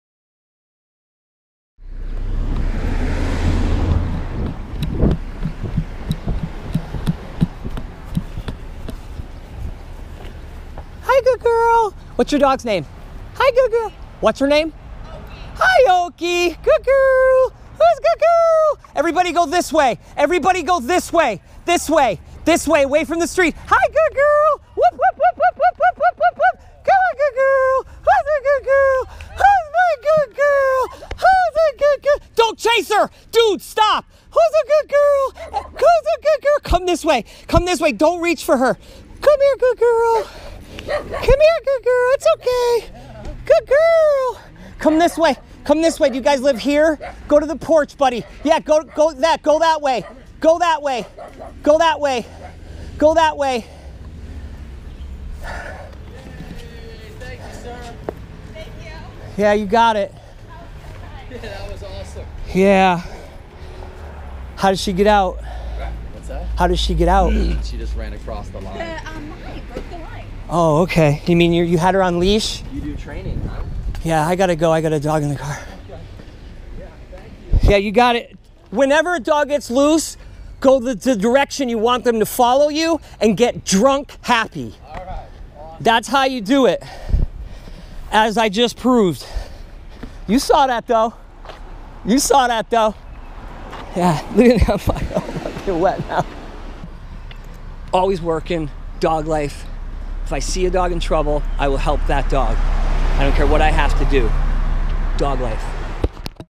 OFF LEASH DOG RESCUE JUNE 8 2021 RUMSON
When there is a dog off leash that needs to get to safety, the humans need to get really happy, like drunk happy, and move in the direction they want to dog to go.
DO NOT SCOLD
DO NOT CHASE
DO NOT ABRUPTLY REACH

This video is an example of how to get an off leash dog to safety.
Drayton Michaels, CTC June 8 2021